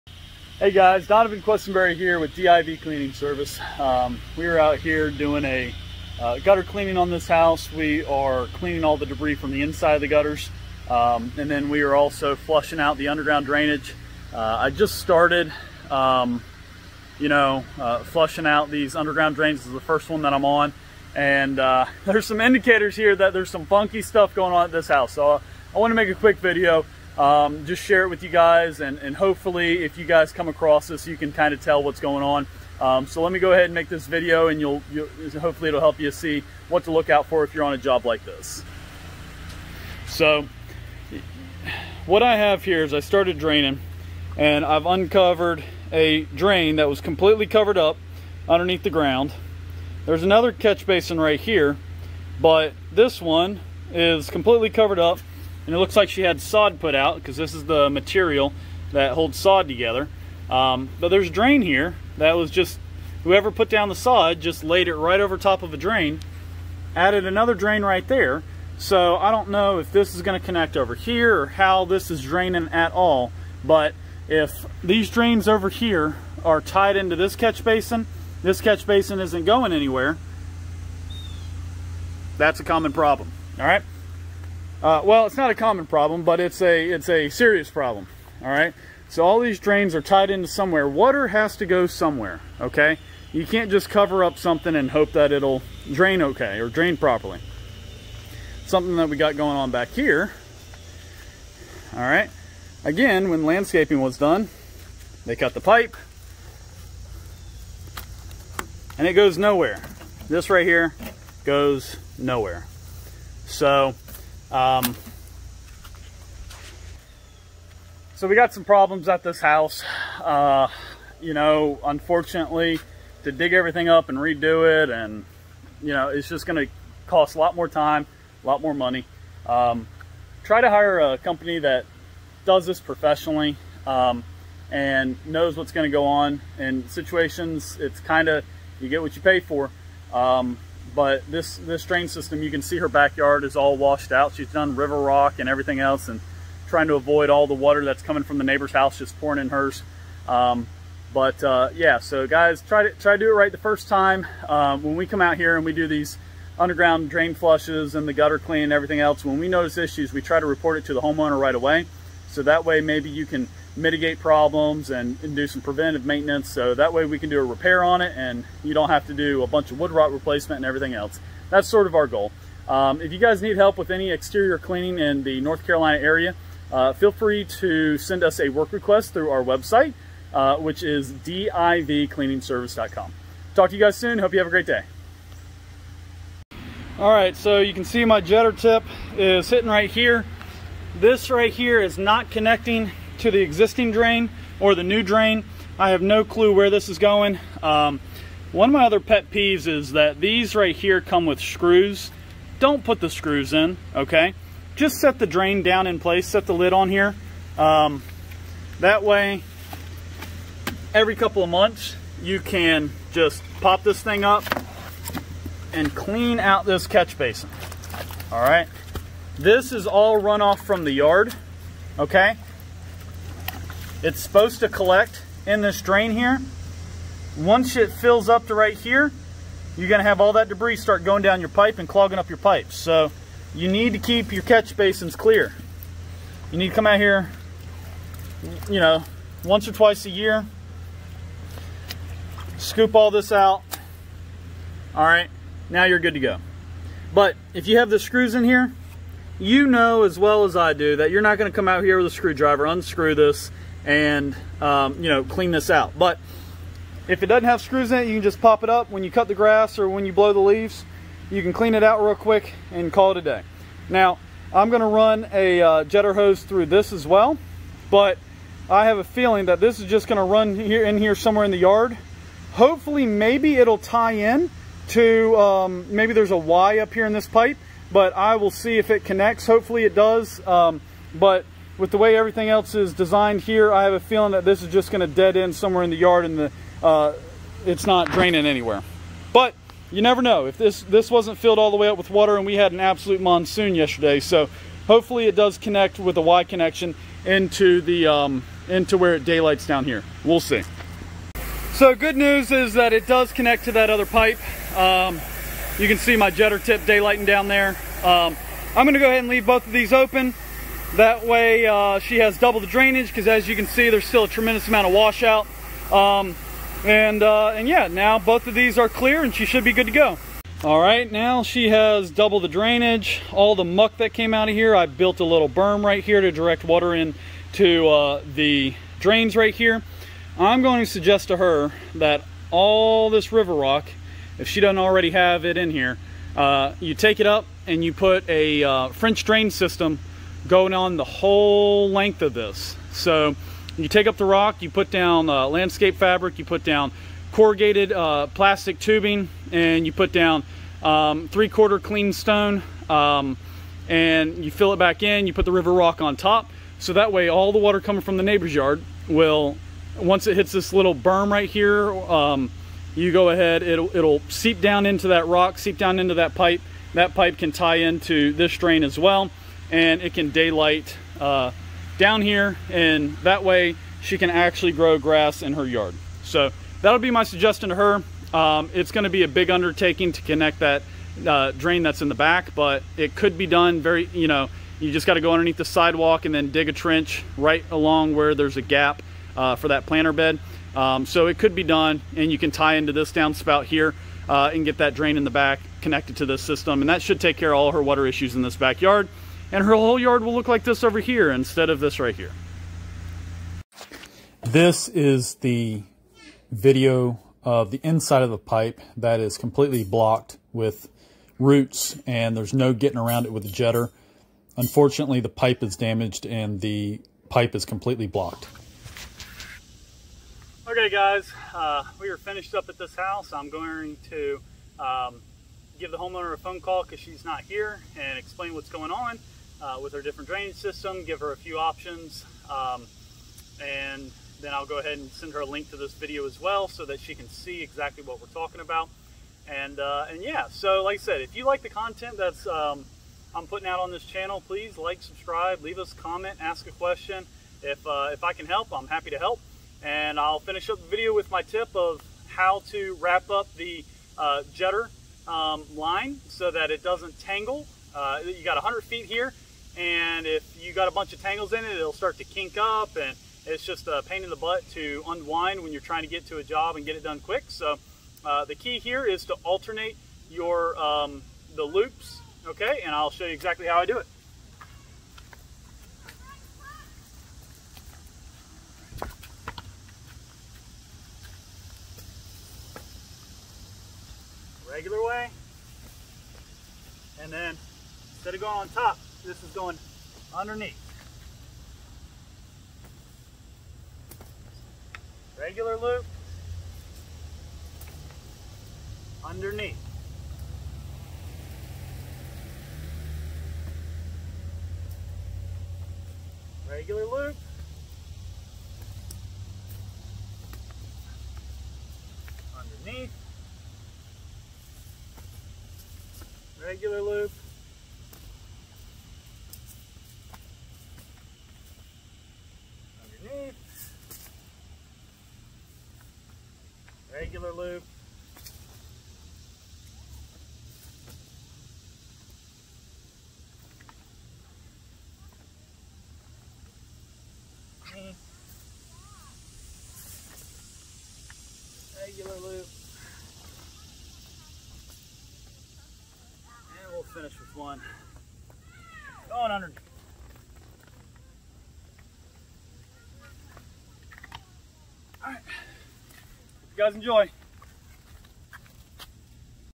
Landscaper destroyed this clients underground drain system
Landscapers cover up a ground catch basin drain with sod, cut a drainage pipe, and damage a 3rd pipe that now has roots growing in it.

SOFTWARE I use to run my business: Quoting, Invoicing, Routing, CRM, Time/Expense Tracking, CC Processing, and more -